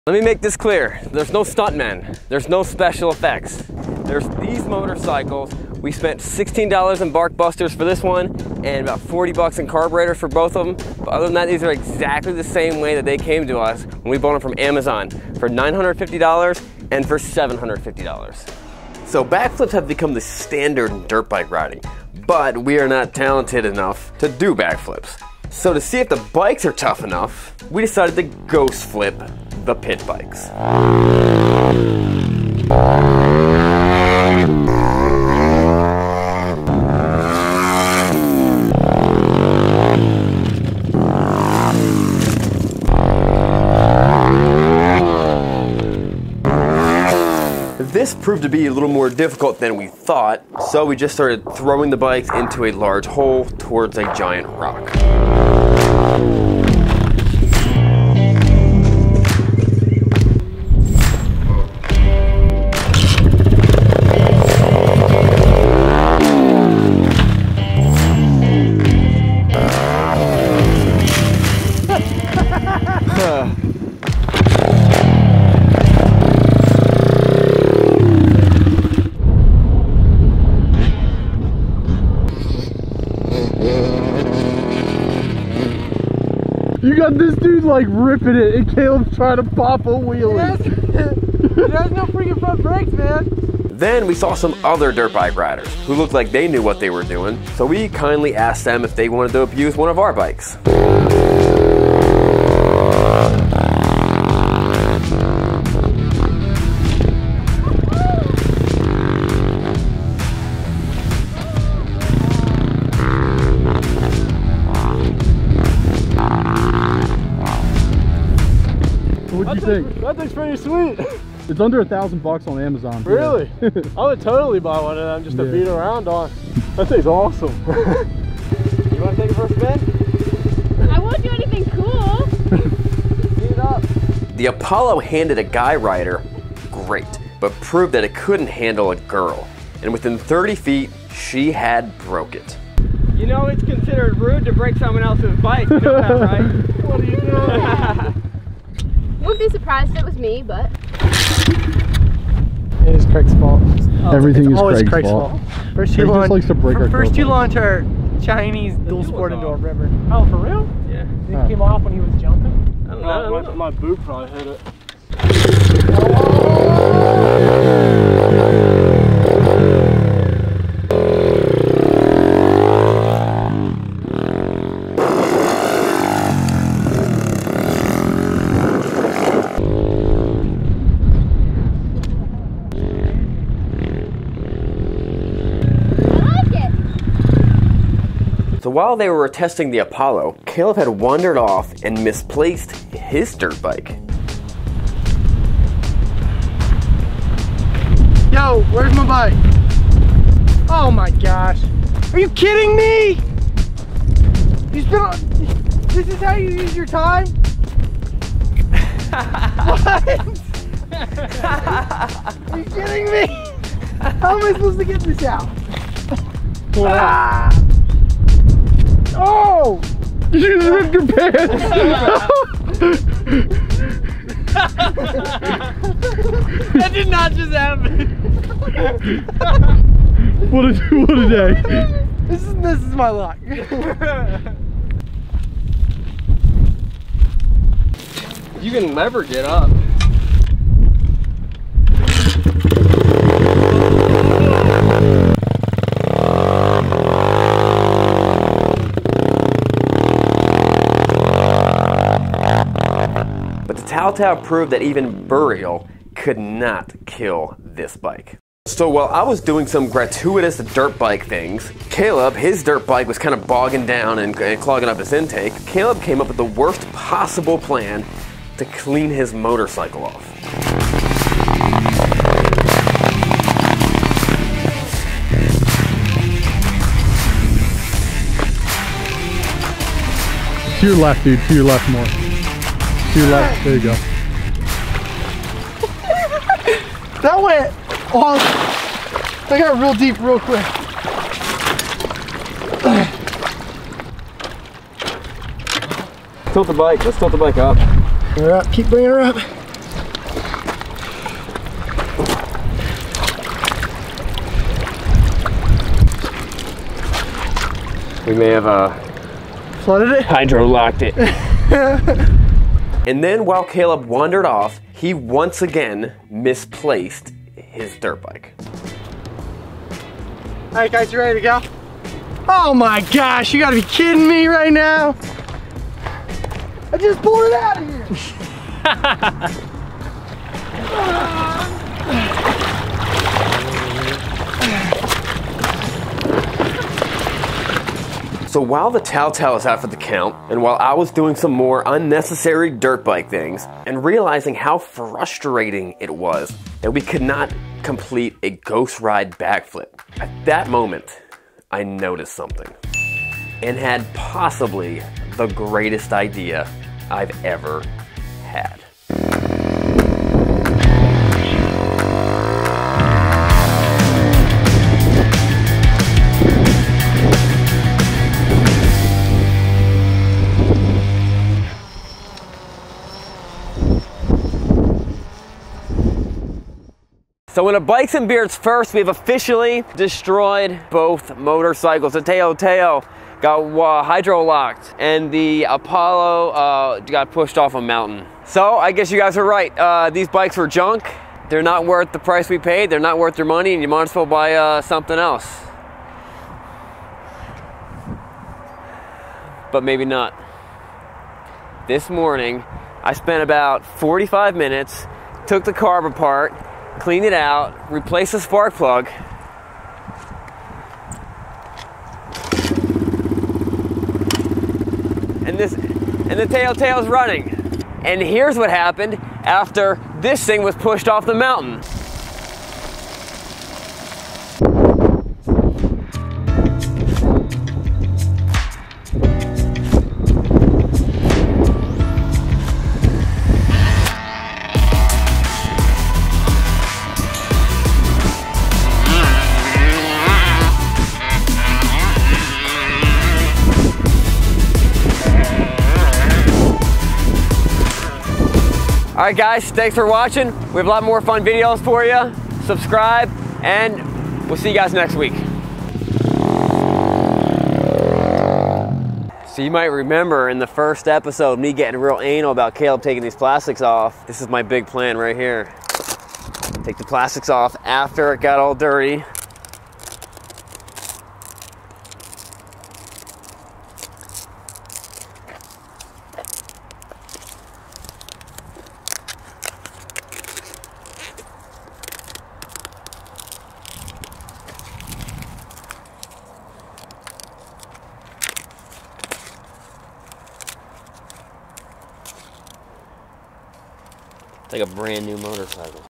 Most Brutal Amazon Bike TORTURE Test EVER!
We put our Amazon dirt bikes through a bunch of brutal torture tests. Will these cheap chinese dirt bikes survive? By the end of the video we decided that the only thing to do was throw the bike off a cliff...